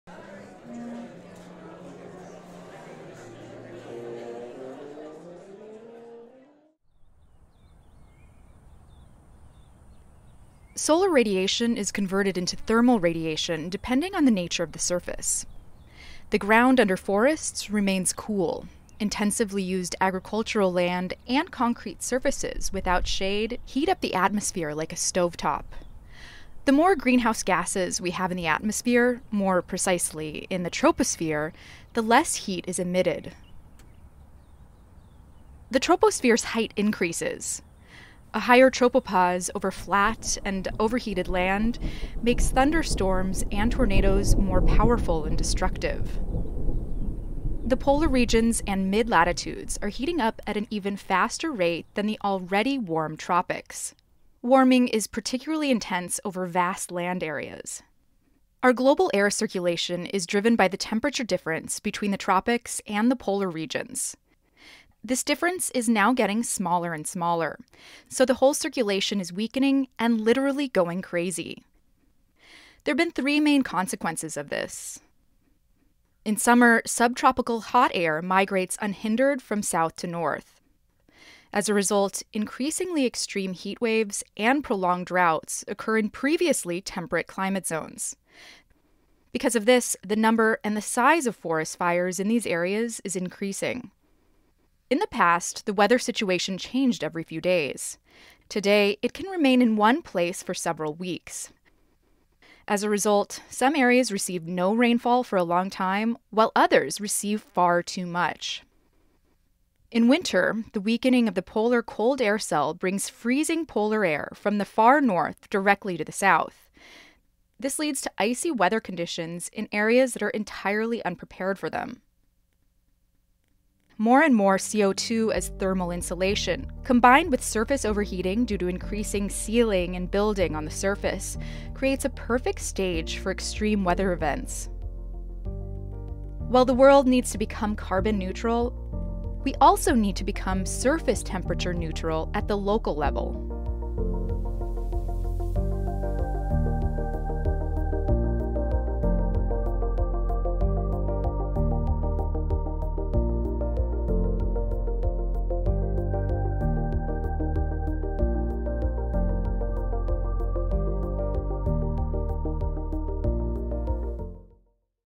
Extreme weather - the perfect stage / part 1
Simple drawn explanation-video
Short: Due to global warming, the global atmospheric circulation is also changing. In combination with the overheating of the surface due to increasing sealing and building development, we are creating the "perfect stage" for extreme weather events.

Idea & Script: Dr. Lucrezia Terzi, Dr. Gerhard Wotawa
Drawings & Editing: Sabine Tschürtz
Speaker: Vanessa Barfield
Music/Special sounds: Coma-Media auf Pixabay/freesound.org
Produced by: Dr. Lucrezia Terzi
Kindly supported by the Austrian Metereological Society
2022

Content: Solar radiation is converted into thermal radiation depending on the nature of the surface. The ground under forests remains cool. Intensively used agricultural land and concreted surfaces without shade heat up the atmosphere like a stovetop. The more greenhouse gases we have in the atmosphere - more precisely in the troposphere - the less heat is emitted.
The troposphere's height increases. A higher tropopause over flat and overheated land makes thunderstorms and tornadoes more powerful and destructive.
The polar regions and mid-latitudes are warming at an even stronger rate than the already warm tropics. Warming is particularly strong over vast land areas.
Our global air circulation is driven by the temperature difference between the tropics and the polar regions. This difference is now getting smaller and smaller - so the whole circulation is weakening and literally goes crazy. This has 3 main consequences:

1.) In summer, subtropical hot air migrates unhindered from south to north. As a result, increasingly extreme heat waves and prolonged droughts occur in previously temperate climate zones. Therefore, the number and the size of forest fires in these areas increases.
2.) In the past, the weather situation changed every few days. Today, it can remain in one place for several weeks. As a result, some areas receive no rainfall for a long time, while others receive far too much.
3.) In winter, the weakening of the polar cold air cell brings freezing polar air from the far north directly to the south. This leads to icy weather conditions in areas entirely unprepared for them.

More and more CO2 as "thermal insulation" combined with surface overheating due to increasing sealing and building on the surface creates a perfect stage for extreme weather events. While the world needs to become "carbon neutral", we need to become "surface temperature neutral" at the local level.